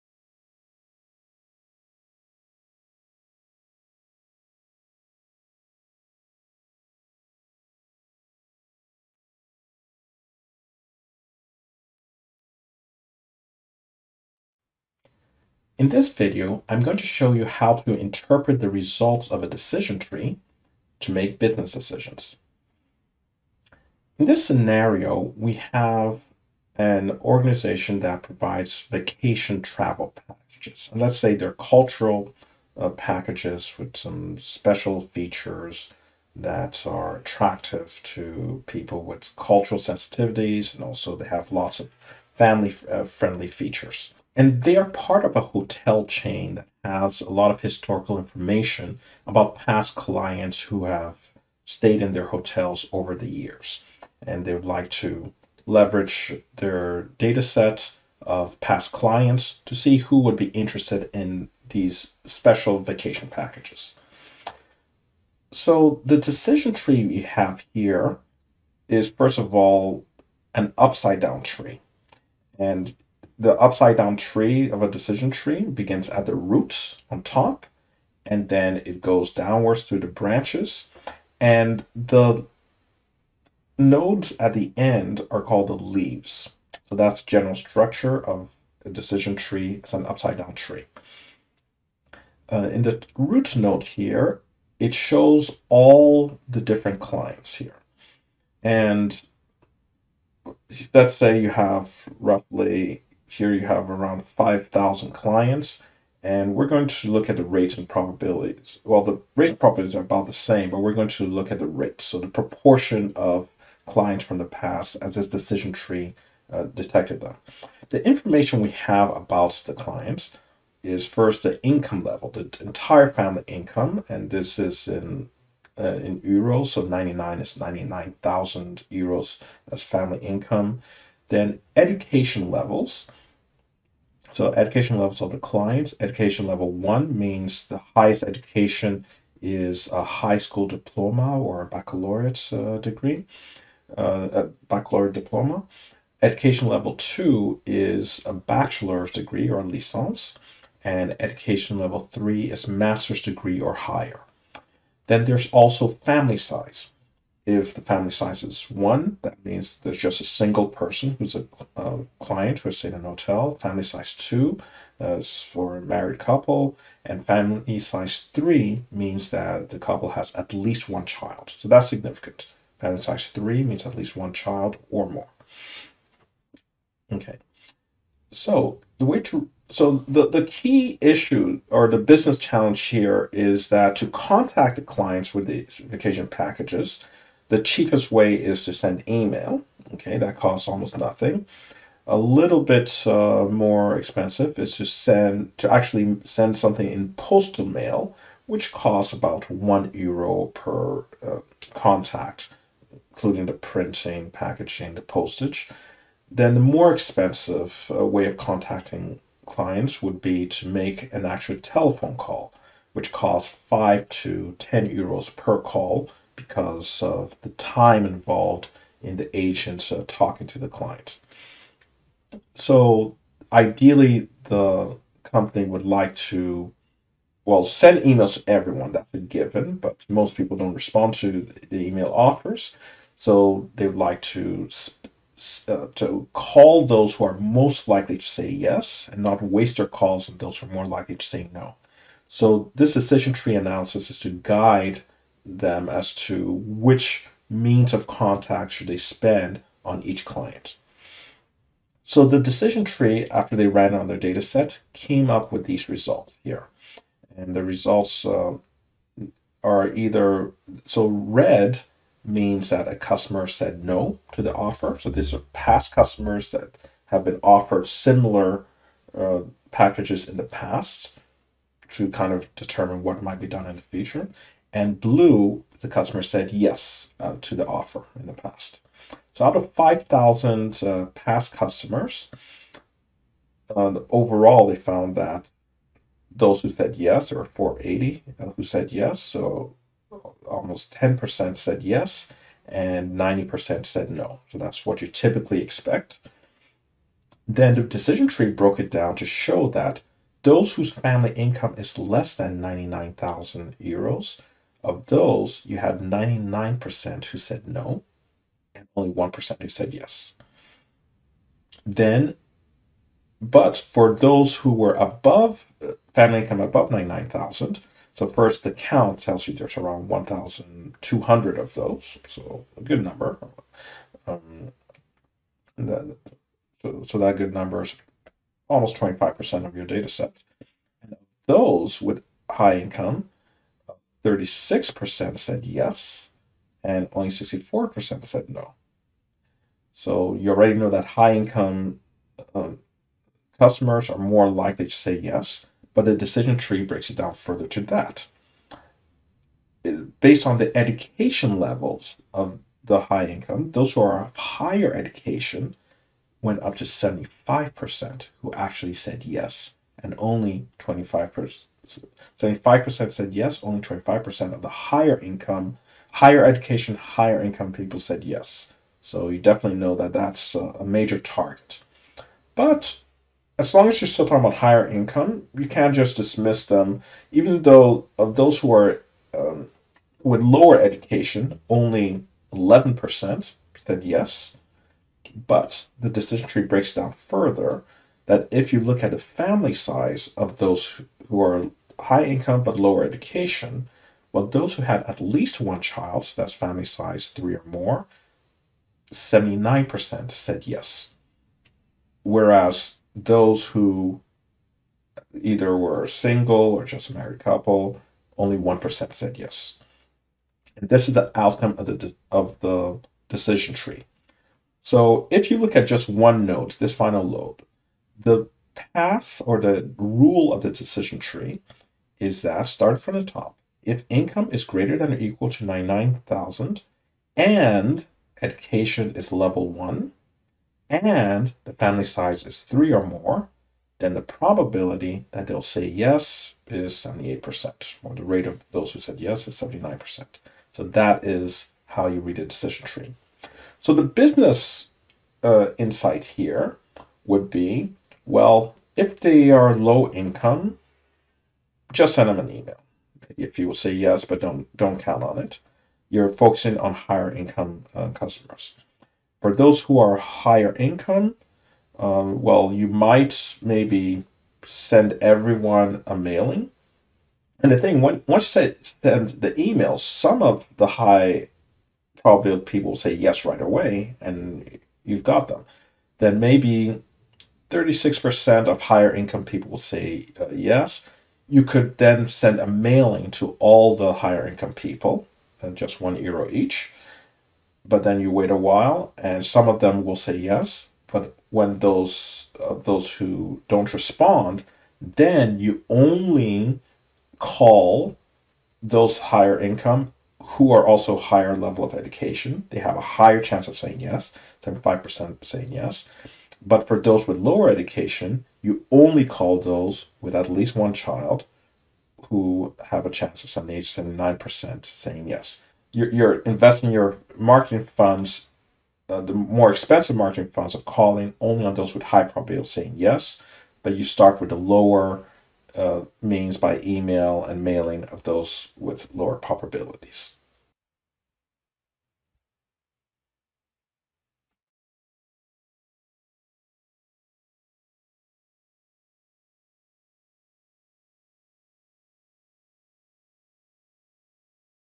Business Application of Decision Trees - Classification - Business Intelligence with Data Mining
An example of how a decision tree can be interpreted to make decisions for optimizing a marketing campaign